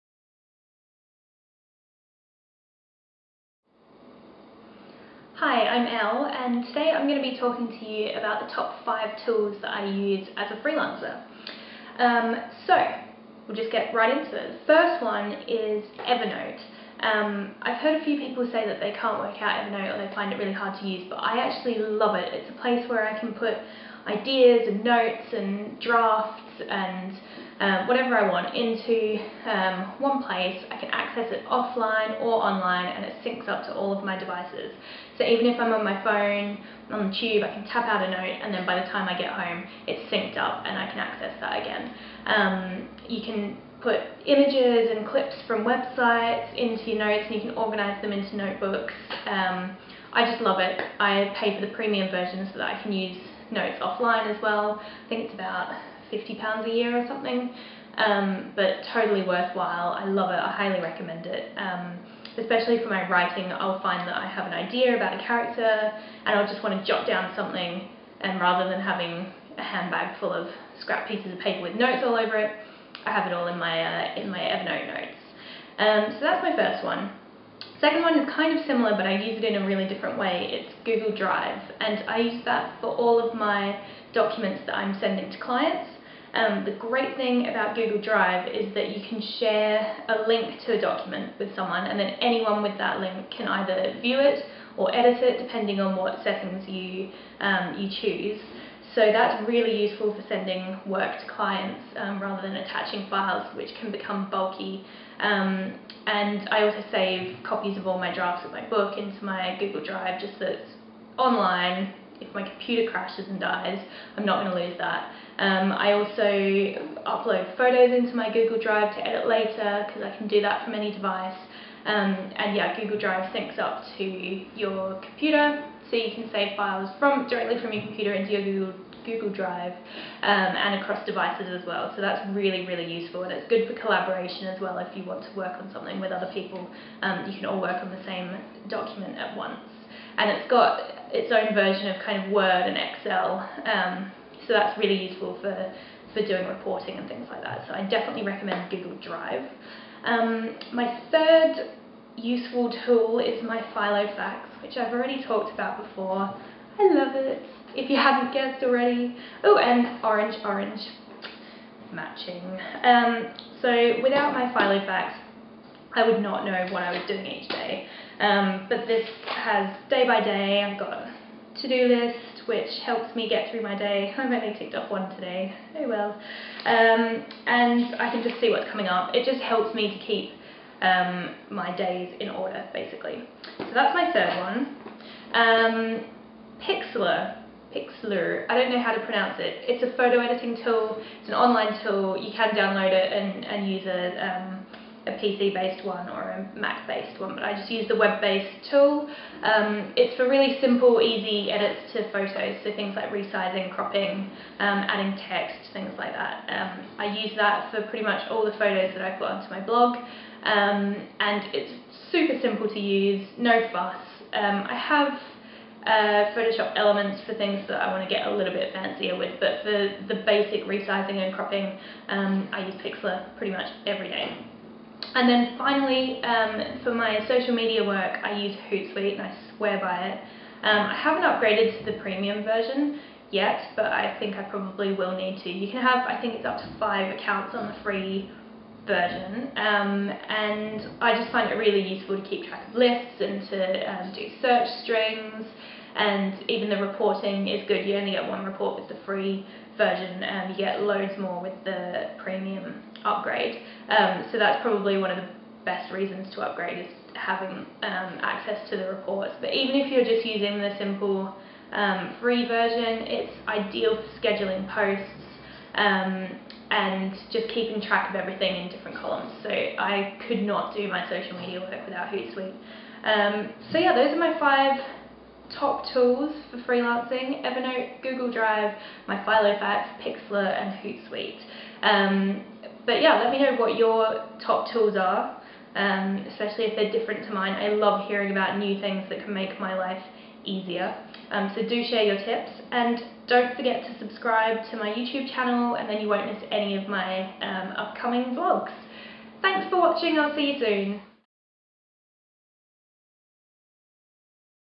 My Freelance Life - Top 5 Tools
I share with you my top five tools for freelancing, including social media management, document collaboration, photo editing and more!